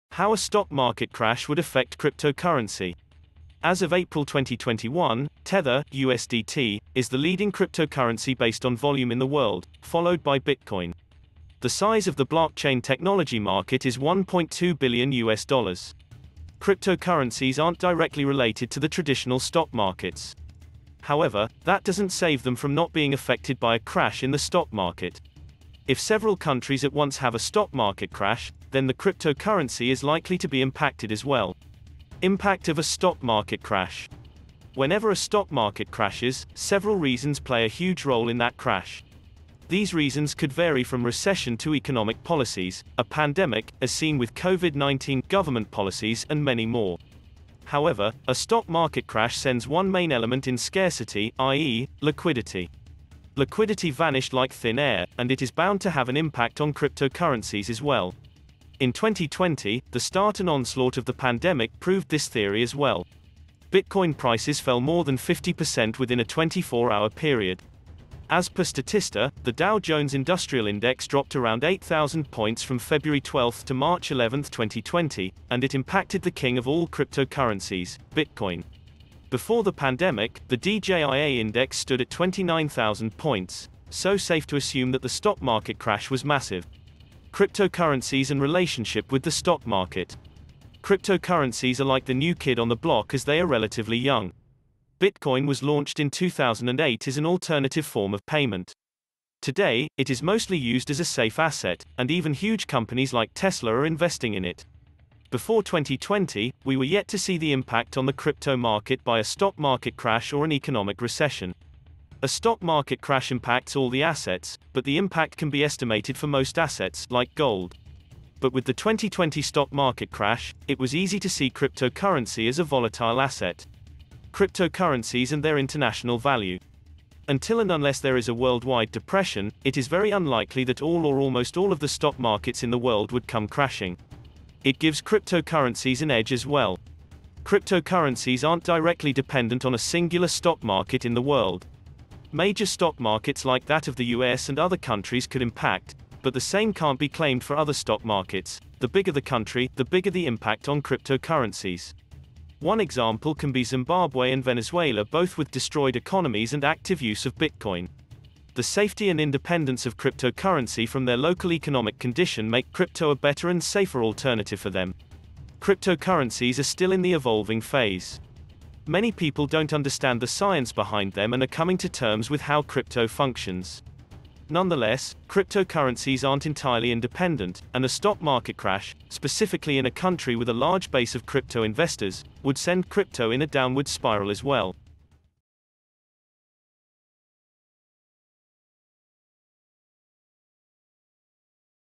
How a Stock Market Crash Would Affect Crypto Currency | Shifu Digital
In this video, we are going to talk about How a Stock Market Crash Would Affect CryptoCurrency.
As of April 2021, Tether (USDT) is the leading cryptocurrency based on volume in the world, followed by bitcoin. The size of the blockchain technology market is 1.2 Billion US Dollars. Cryptocurrencies aren't directly related to the traditional stock markets. However, that doesn't save them from not being affected by a crash in the stock market. If several countries at once have a stock market crash, then the cryptocurrency is likely to be impacted as well.

Impact of a stock market crash:
Whenever a stock market crashes, several reasons play a huge role in that crash. These reasons could vary from recession to economic policies, a pandemic (as seen with Covid-19), government policies, and many more. However, a stock market crash sends one main element in scarcity, i.e., liquidity. Liquidity vanished like thin air, and it is bound to have an impact on cryptocurrencies as well.

In 2020, the start and onslaught of the pandemic proved this theory as well. Bitcoin prices fell more than 50% within a 24 hour period. As per Statista, the Dow Jones Industrial Index dropped around 8000 points from February 12 to March 11, 2020, and it impacted the king of all cryptocurrencies – Bitcoin. Before the pandemic, the DJIA index stood at 29,000 points - so safe to assume that the stock market crash was massive.

Cryptocurrencies and relationship with the stock market:
Cryptocurrencies are like the new kid on the block as they are relatively young. Bitcoin was launched in 2008 as an alternative form of payment. Today, it is mostly used as a safe asset, and even huge companies like Tesla are investing in it. Before 2020, we were yet to see the impact on the crypto market by a stock market crash or an economic recession. A stock market crash impacts all the assets, but the impact can be estimated for most assets, like gold. But with the 2020 stock market crash, it was easy to see cryptocurrency as a volatile asset.

Cryptocurrencies and their international value:
Until and unless there is a worldwide depression, it is very unlikely that all or almost all of the stock markets in the world would come crashing. It gives cryptocurrencies an edge as well. Cryptocurrencies aren't directly dependent on a singular stock market in the world. Major stock markets like that of the US and other countries could impact, but the same can't be claimed for other stock markets – the bigger the country, the bigger the impact on cryptocurrencies.

One example can be Zimbabwe and Venezuela- both with destroyed economies and active use of bitcoin. The safety and independence of cryptocurrencies from their local economic condition make crypto a better and safer alternative for them.

Cryptocurrencies are still in the evolving phase. Many people don't understand the science behind them and are coming to terms with how crypto functions. Nonetheless, cryptocurrencies aren't entirely independent, and a stock market crash, specifically in a country with a large base of crypto investors, would send crypto in a downward spiral as well.

Write in the comments what the video looks like and what region of the world you are watching from.